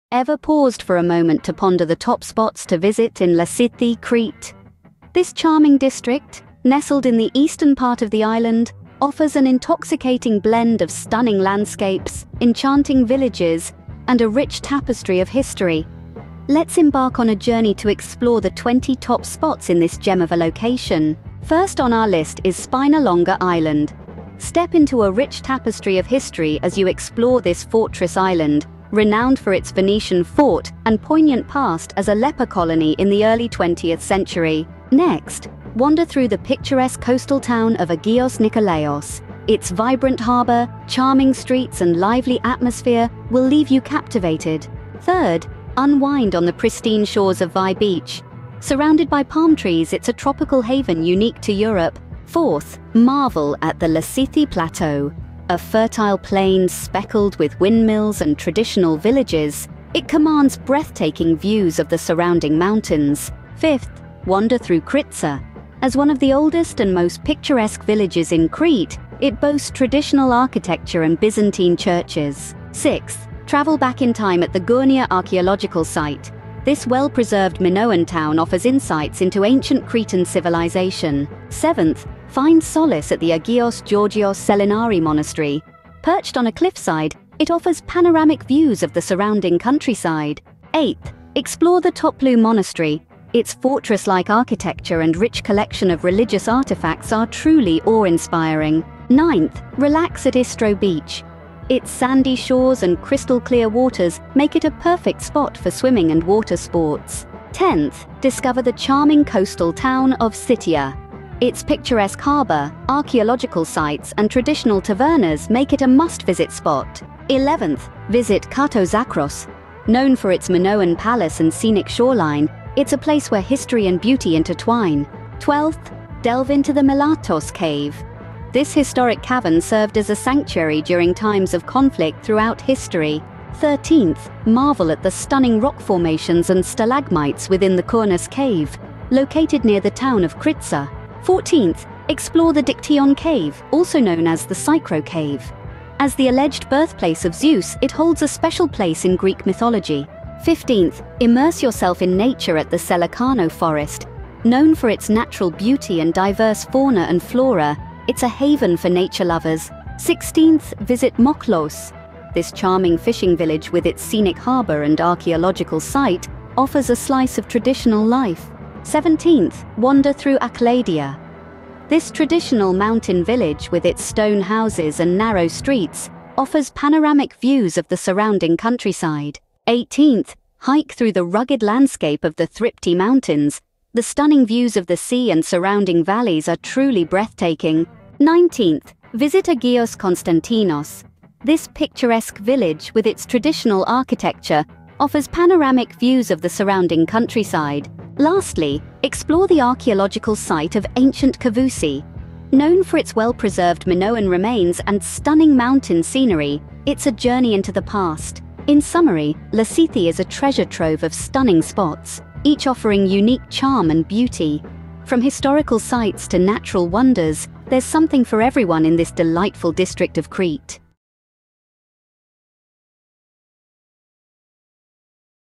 LASITHI: Some Places to Visit
Lasithi is a beautiful region located in the eastern part of the island of Crete, Greece, known for its stunning landscapes, charming villages, and rich history. Here are 20 top places to visit in Lasithi:

    Spinalonga Island: Explore the historic fortress island, known for its Venetian fortress and its use as a leper colony in the early 20th century.

    Agios Nikolaos: Visit this picturesque coastal town known for its scenic harbor, charming streets, and vibrant atmosphere.

    Vai Beach: Relax on the pristine beach surrounded by palm trees, unique to Europe.

    Lasithi Plateau: Discover this fertile plain dotted with windmills and traditional villages, offering stunning views of the surrounding mountains.

    Kritsa: Explore one of the oldest and most picturesque villages in Crete, known for its traditional architecture and Byzantine churches.

    Gournia Archaeological Site: Step back in time at this well-preserved Minoan town, offering insights into ancient Cretan civilization.

    Agios Georgios Selinari Monastery: Visit this serene monastery perched on a cliffside, offering panoramic views of the surrounding countryside.

    Toplou Monastery: Explore this impressive monastery with its fortress-like architecture and rich collection of religious artifacts.

    Istro Beach: Relax on the sandy beach with crystal-clear waters, ideal for swimming and water sports.

    Sitia: Discover this charming coastal town with its picturesque harbor, archaeological sites, and traditional tavernas.

    Kato Zakros: Visit the archaeological site and beach at Kato Zakros, known for its Minoan palace and scenic coastline.

    Milatos Cave: Explore this historic cave, which served as a place of refuge during times of conflict throughout history.

    Kournas Cave: Discover the stunning stalactites and stalagmites inside this cave, located near the village of Kritsa.

    Dikteon Cave (Psychro Cave): Explore the legendary birthplace of Zeus, the king of the Greek gods, located high in the Dikti Mountains.

    Selakano Forest: Hike through this pristine forest, known for its natural beauty and diverse flora and fauna.

    Mochlos: Visit this charming fishing village with its scenic harbor and archaeological site, located on a small island off the coast.

    Achladia: Explore this traditional mountain village with its stone houses, narrow streets, and panoramic views of the surrounding countryside.

    Thripti Mountains: Hike through the rugged landscape of the Thripti Mountains, offering stunning views of the sea and surrounding valleys.

    Agios Konstantinos: Visit this picturesque village with its traditional architecture and panoramic views of the surrounding countryside.

    Kavousi: Explore the archaeological site of ancient Kavousi, known for its well-preserved Minoan remains and stunning mountain scenery.

These are just a few of the many incredible places to visit in Lasithi, each offering its own unique charm and beauty.